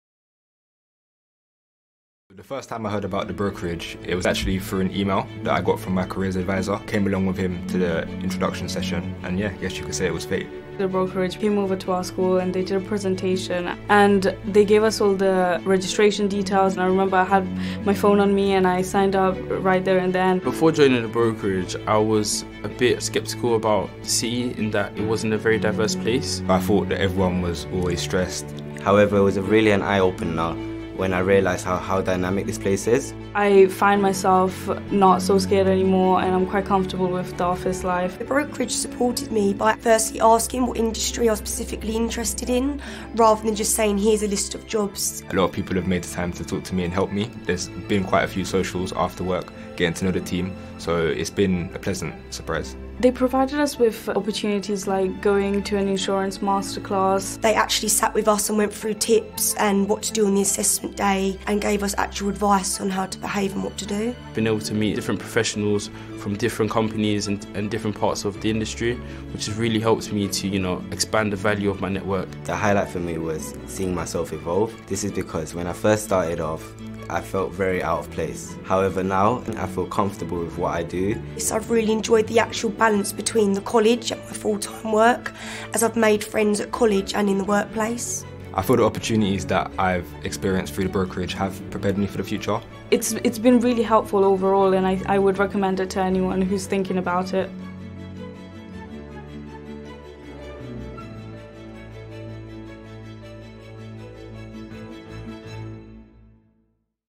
Are you 16 - 25 and looking for a professional career, but lack the networks to get you in?
The Brokerage is a charity waiting to help you to:
- Grow your networks
- Enhance your skills
- Help you build a winning CV
- Invite you to exclusive masterclasses and events.